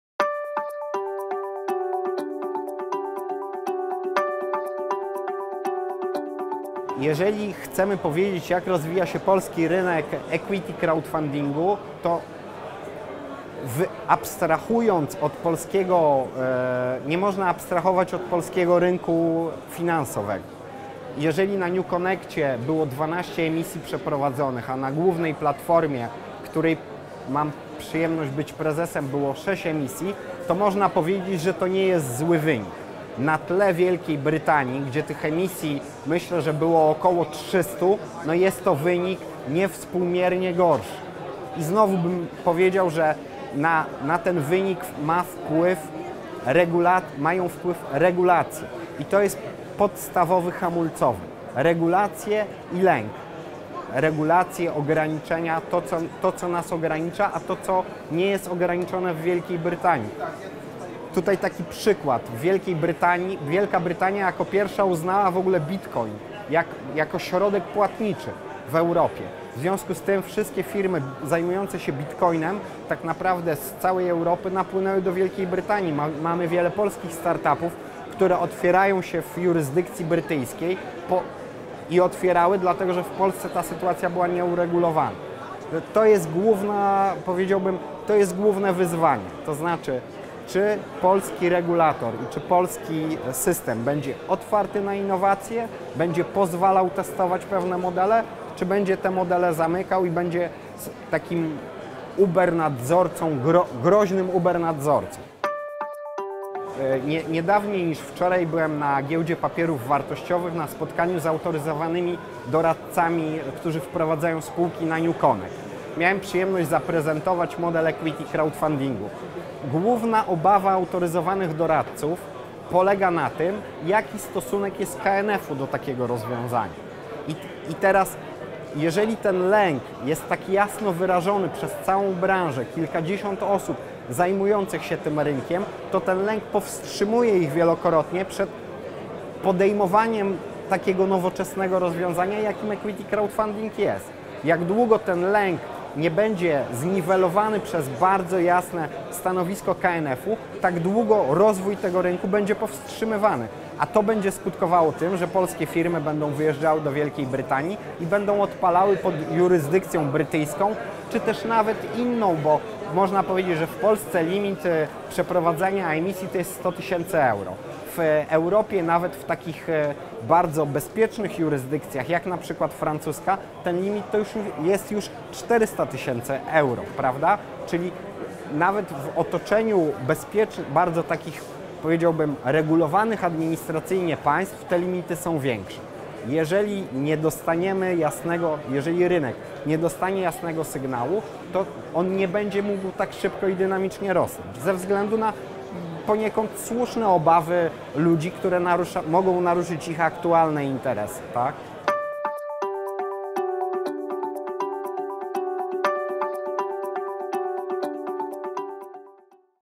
impact'16 fintech/insurtech: Arkadiusz Regiec, Beesfund SA, część 2
Arkadiusz Regiec, Prezes Zarządu Beesfund SA o polskim rynku equity crowdfundingu, o naszych ograniczeniach oraz o tym, jak to się robi w Unii Europejskiej.